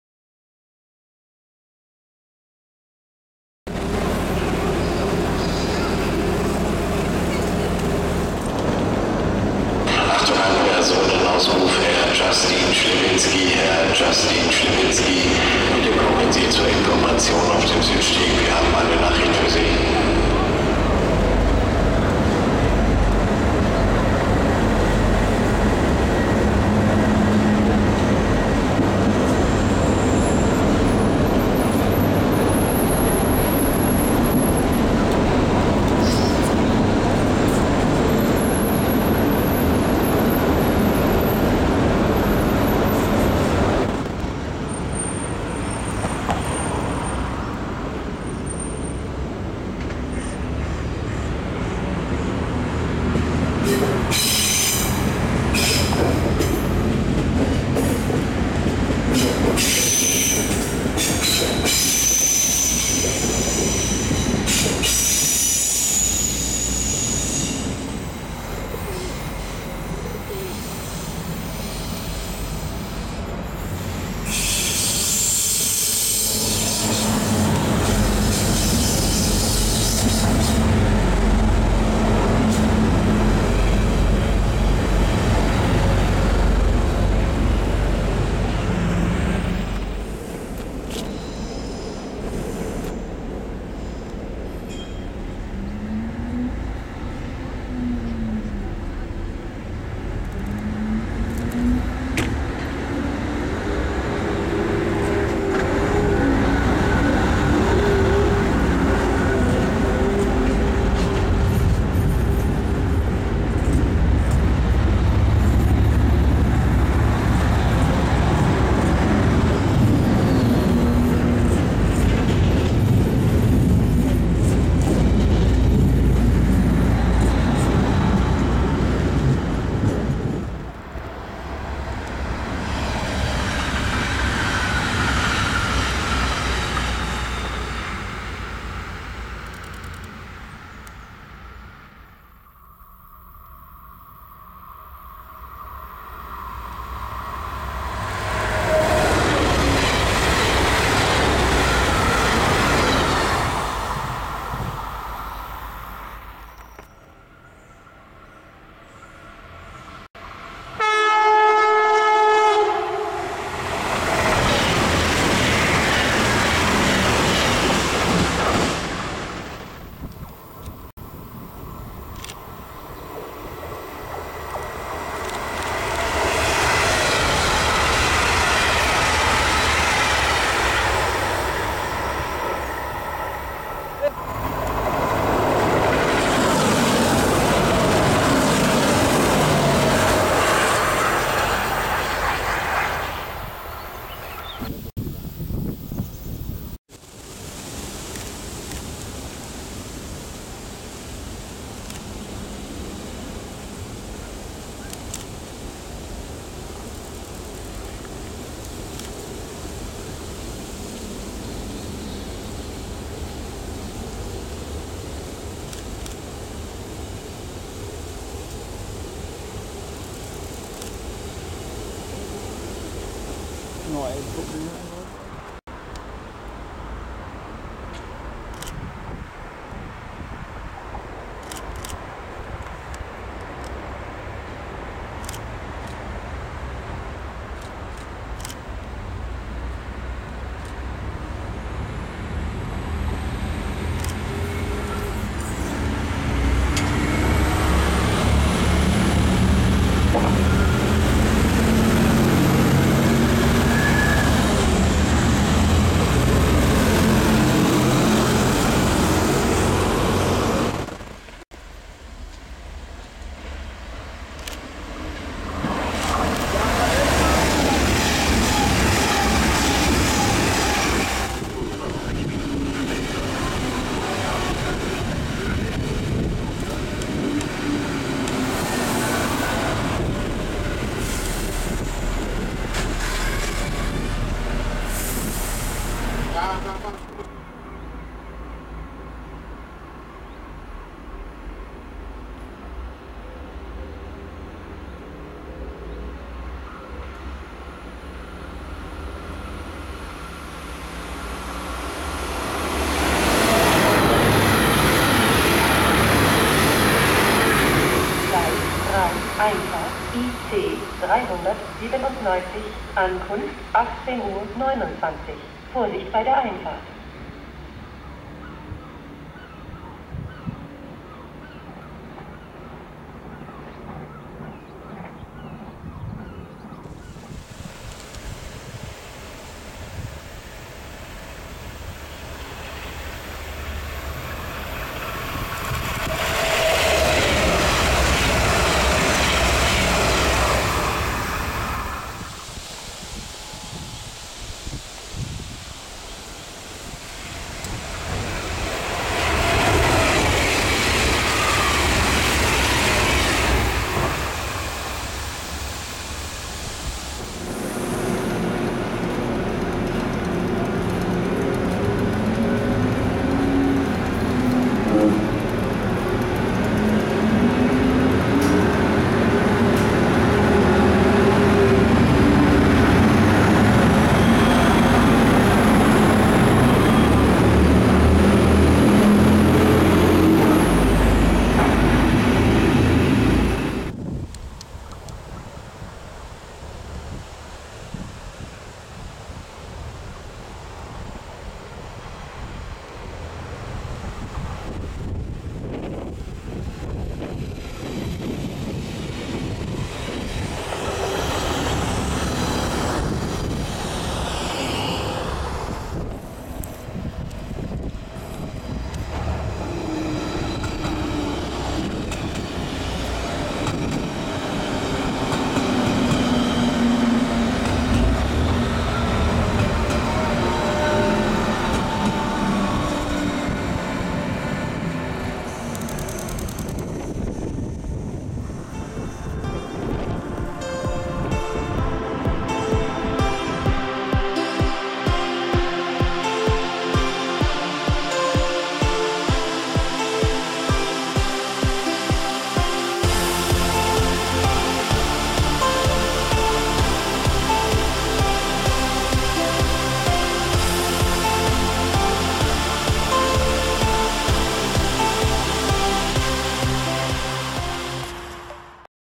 IC3 i Schleswig-Holstein og Hamborg [2022]
Igen i år satte jeg kursen mod syd for at fotografere DSBs IC3-tog i Tyskland, da dette muligvis er sidste sommer IC3 i den internationale trafik mellem Hamborg og Danmark. I år prøvede jeg nogle andre vinkler og steder end sidste år.
Videoklippene er optaget i juli 2022.

Med på turen var bl.a. @sepper707

I denne video ses de danske IC3'ere følgende steder:
00:00 Intro
00:09 Hamburg Hauptbahnhof
01:08 Hamburg Dammtor
01:30 Hamburg-Altona
02:11 Halstenbek
02:37 Pinneberg
02:50 Horst (Holstein)
03:18 Højbroen i Rendsburg
03:45 Rendsburg
04:21 Owschlag
04:32 Schleswig
05:27 Jübek
05:58 Flensburg
06:24 Grænsen DK/DE (Ved Padborg)
06:47 Padborg